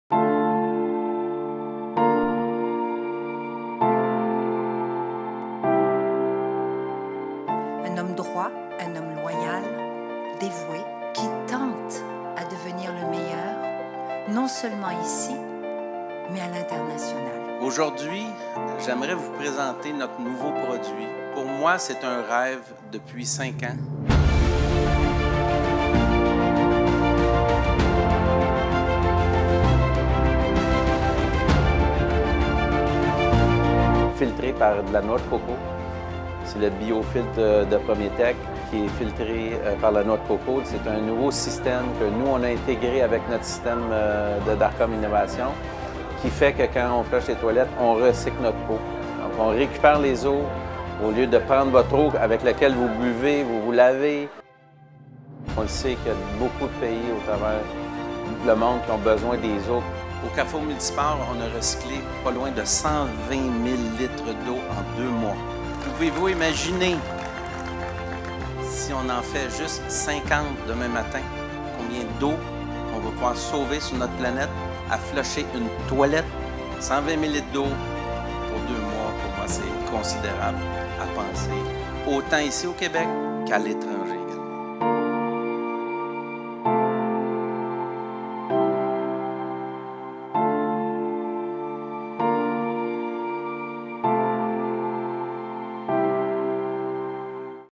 Darcom Innovations inc. - Conteneurs transformables - Lancement du bloc sanitaire écologique
Darcom transforme les conteneurs maritimes, recyclés ou neufs,
en espaces utilitaires. Son dernier né, une toilette écologique et autonome en eau courante.

Le BLOC SANITAIRE ÉCOLOGIQUE est révolutionnaire grâce à sa technologie d’assainissement autonome. Les eaux usées sont filtrées et recyclées sans énergie et sans aucune odeur grâce à une biofiltration composée exclusivement d’écorce de noix de coco.

Lancement le 12 juillet 2017 à Boisbriand (Québec)